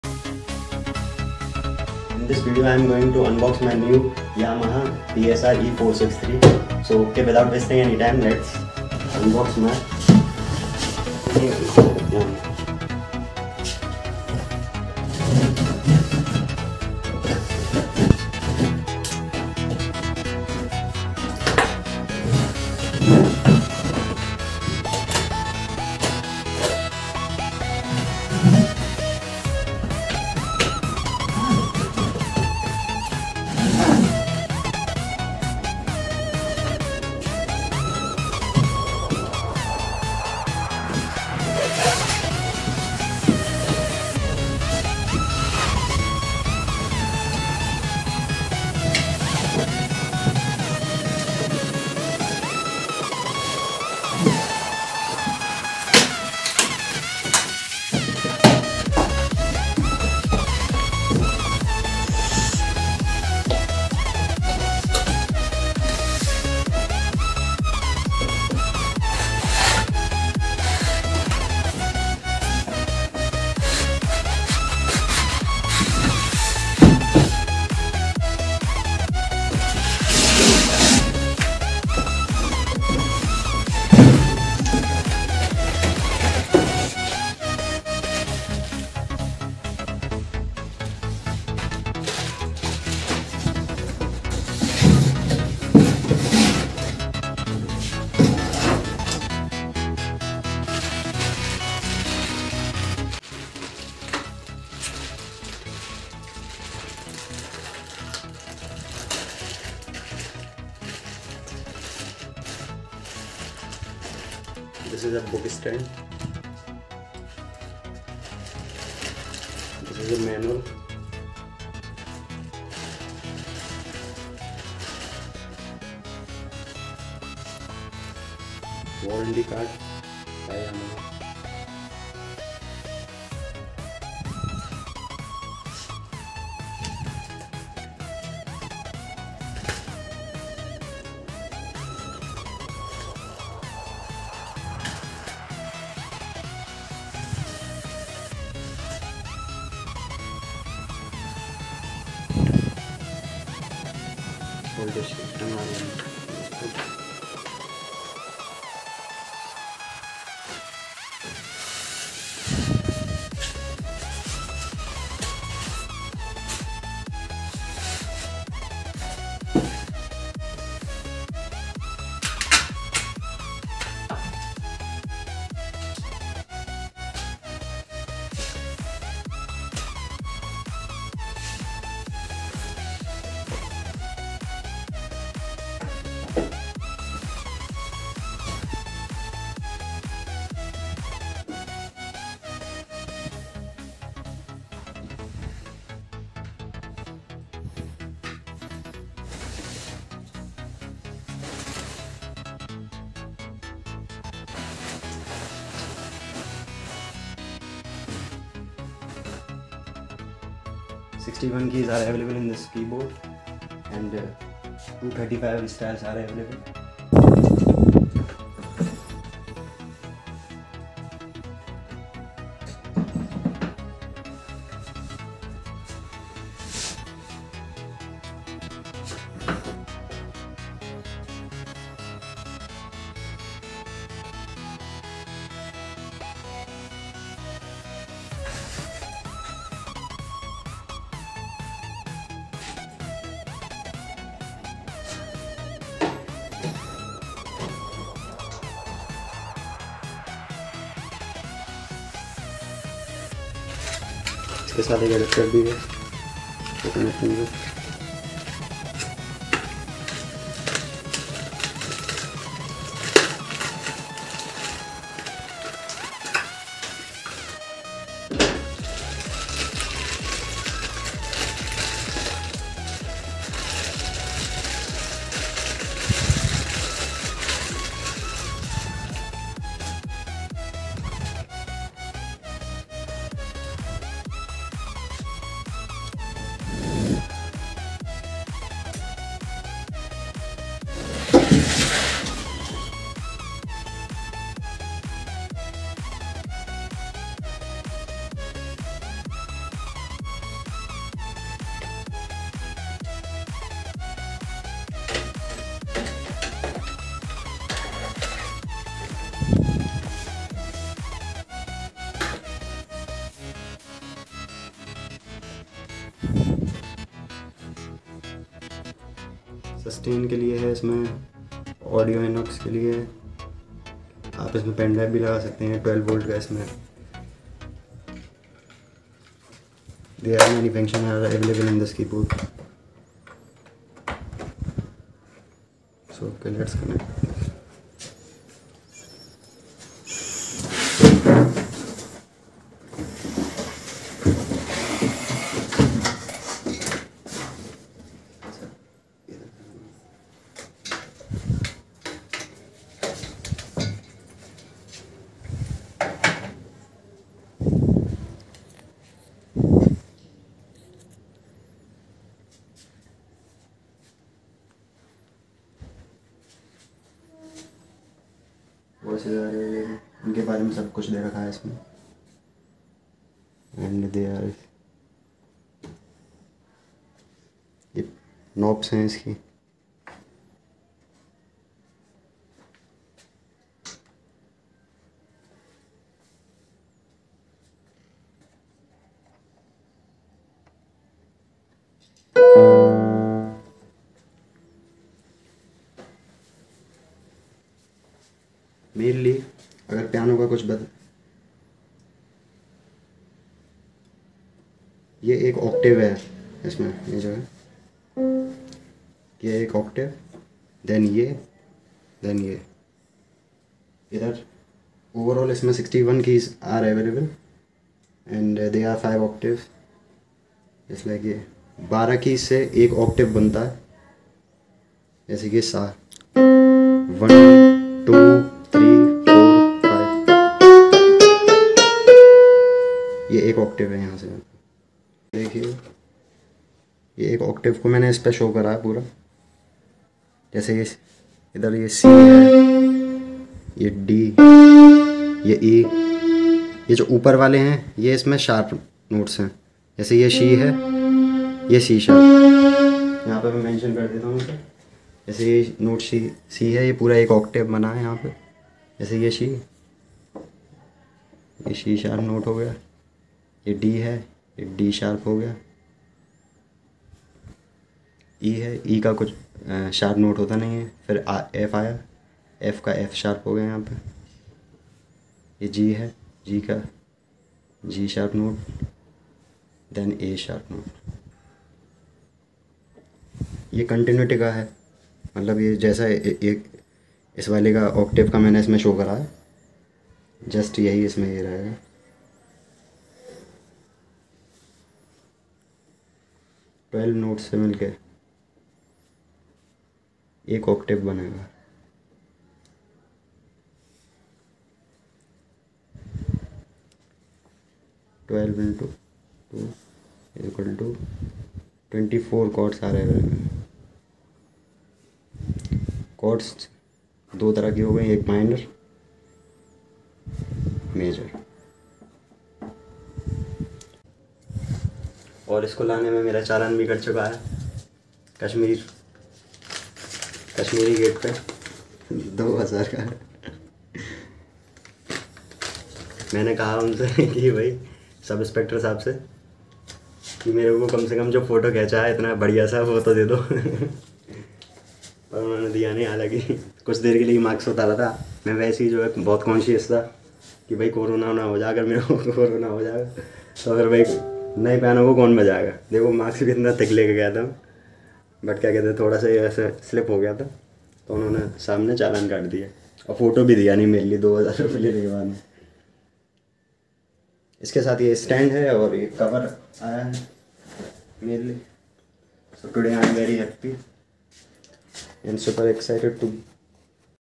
Yamaha PSR e 463 | full Unboxing and basics | Beginner keyboard tutorial | Review | Sagar Chauhan
Hello friends I m SaGar Chauhan in this video I m unboxing my new Yamaha PSR e 463 and Give some basic details about this keyboard 😊
Basic tutorial for beginner -

Skip to  06 : 44 - 08 : 44

I m very happy and excited to unbox this keyboard so here  i m sharing this video with you all
Please increase video quality for proper view and if you want to buy a keyboard then its a good choice for u

Thanks
keep supporting
keep playing 🎶